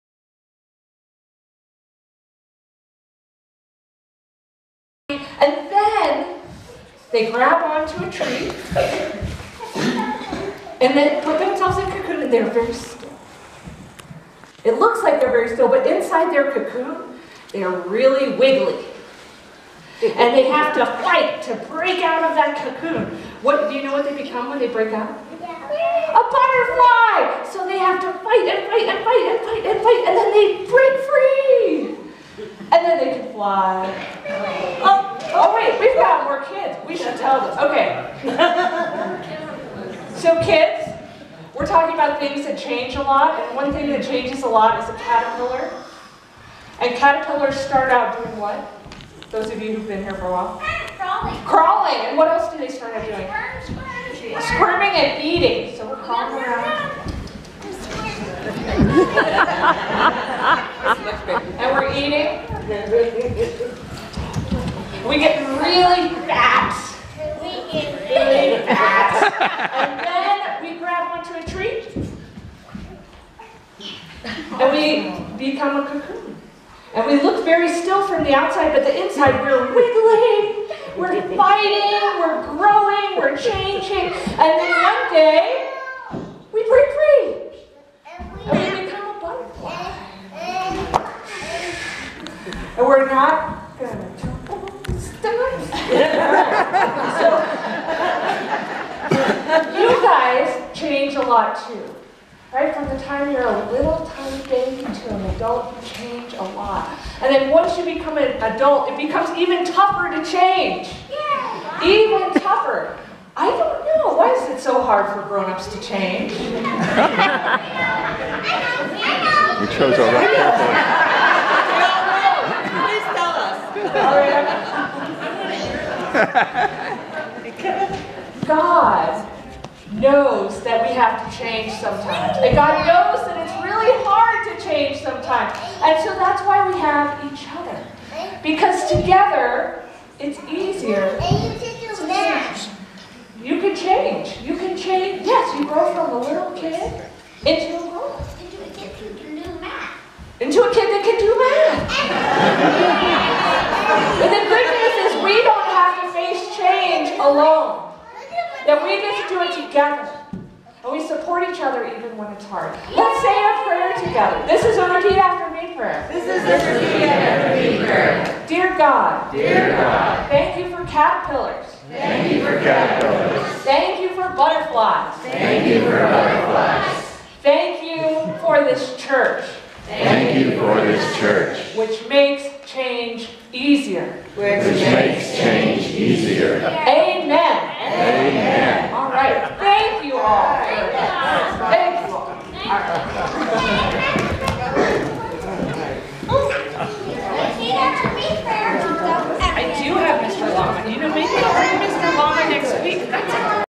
Children Message 2018Feb18
It can be a struggle to CHANGE!  but the children in this video know how to change from a caterpillar to a butterfly.
Church of the Good Shepherd - Children's Message - February 18, 2018